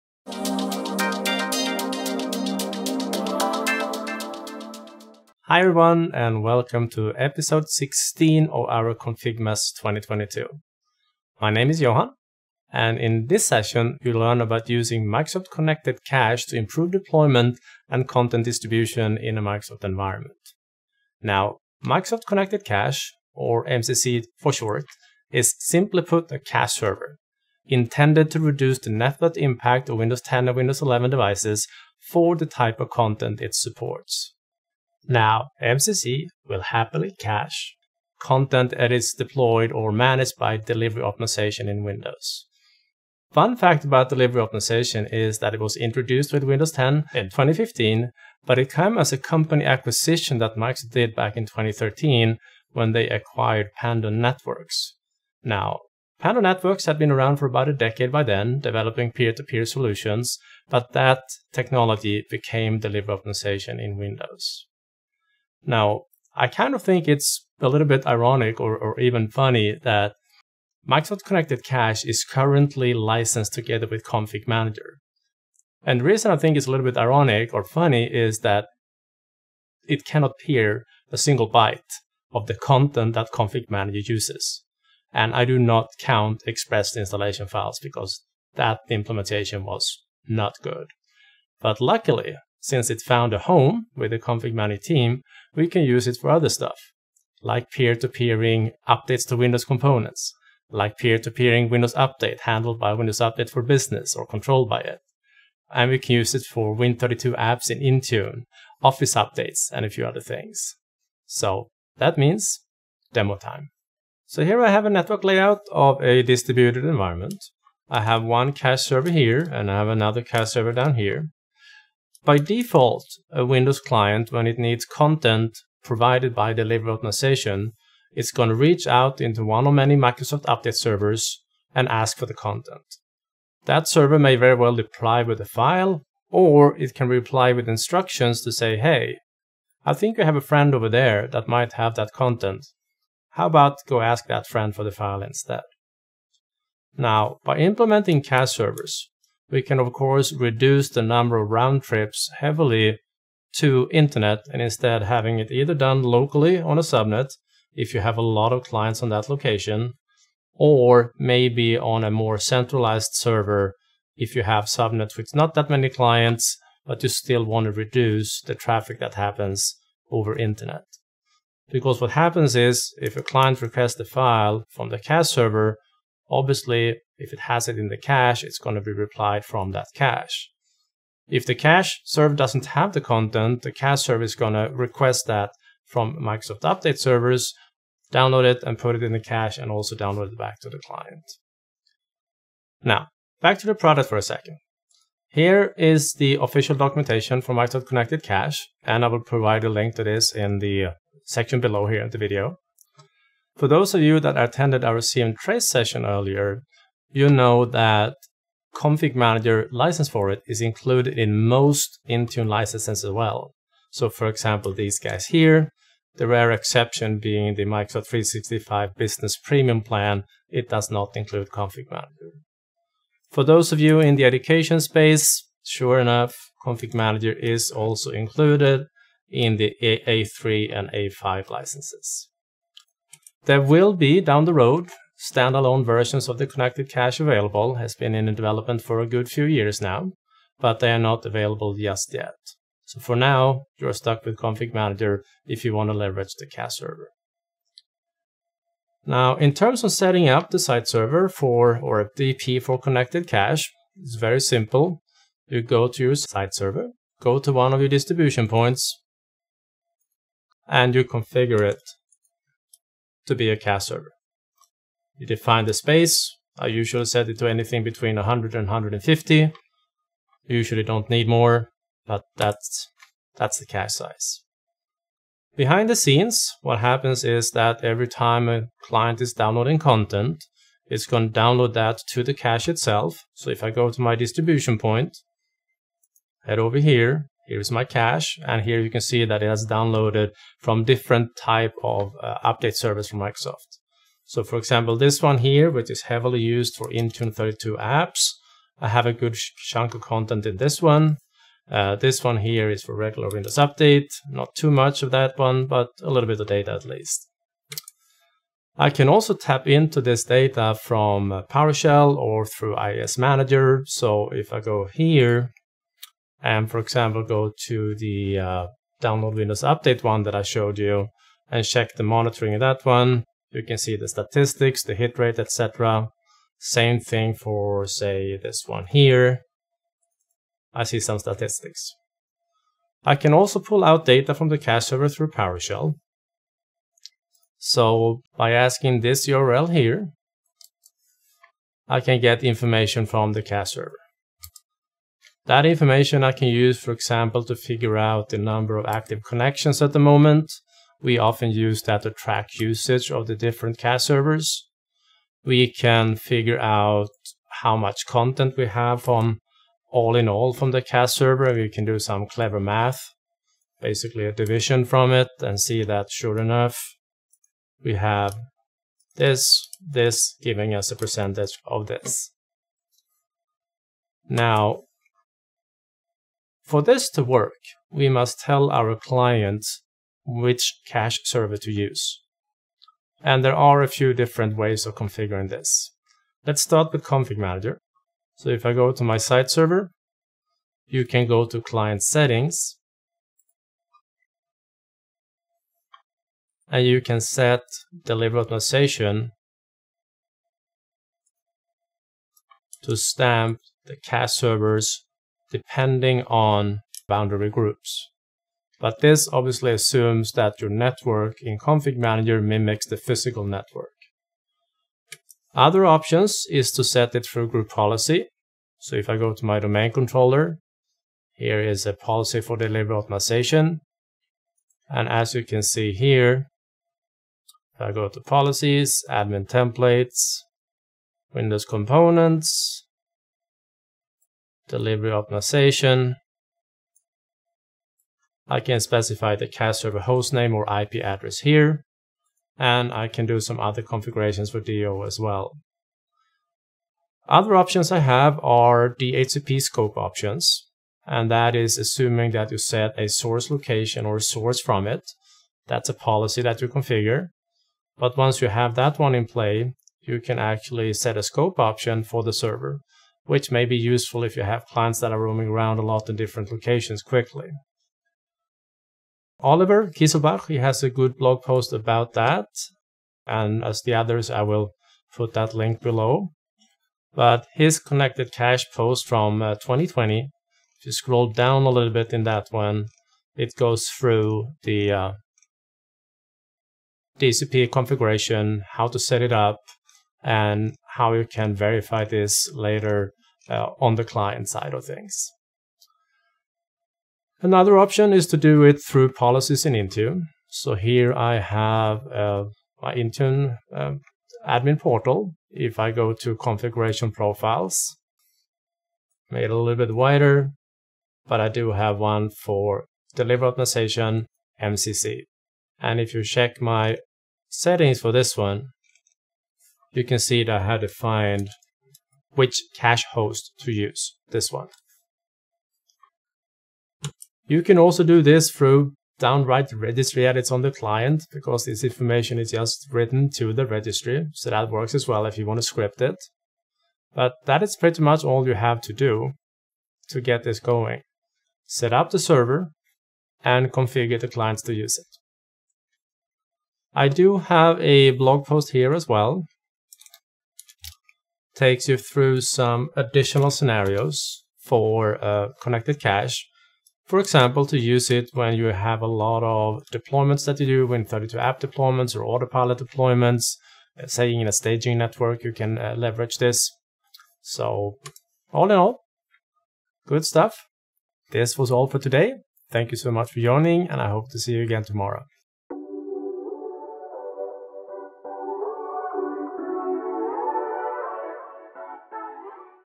ConfigMas 2022 - Episode 16 - Using Microsoft Connected Cache
Demo for Using Microsoft Connected Cache in Microsoft environments.
Please make sure to watch in HD (1080p). Links to the sample scripts and other show-cased resources below:

Delivery Optimization with Intune and Microsoft Connected Cache (MCC)